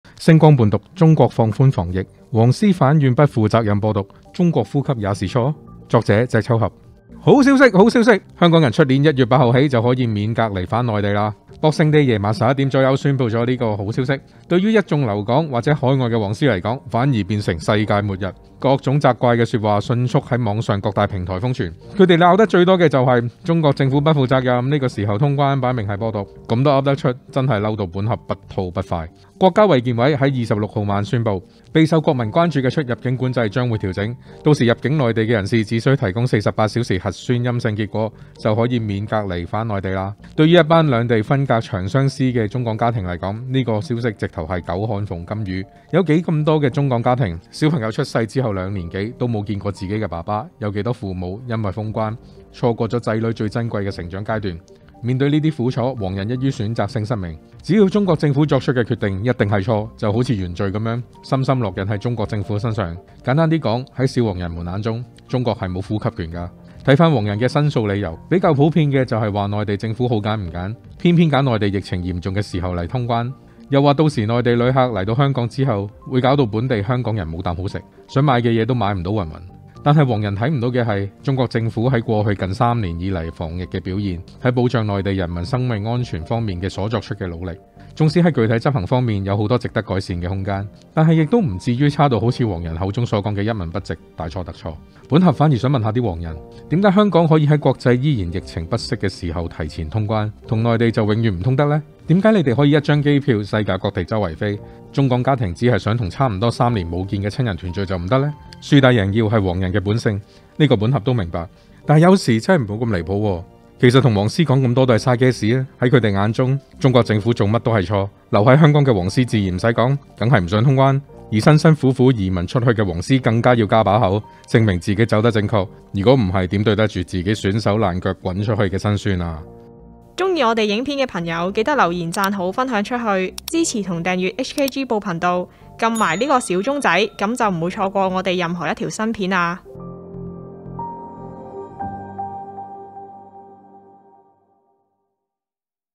聲光伴讀《中國放寬防疫 黃絲反怨不負責任「播毒」 中國呼吸也是錯？》作者：隻抽俠
國家衛健委喺26號晚宣布，備受國民關注嘅出入境管制將會調整，到時入境內地嘅人士只需提供48小時核酸陰性結果，就可以免隔離返內地啦。對於呢個好消息，於一眾留港或海外黃絲嚟講，反而變成「世界末日」。佢哋啲三幅被無非都係：中國政府不負責任，呢個時候通關，擺明係「播毒」云云。都係嘅，如果佢哋唔咁樣講，又點可以證明佢哋乜都啱呢？！

原圖：文匯報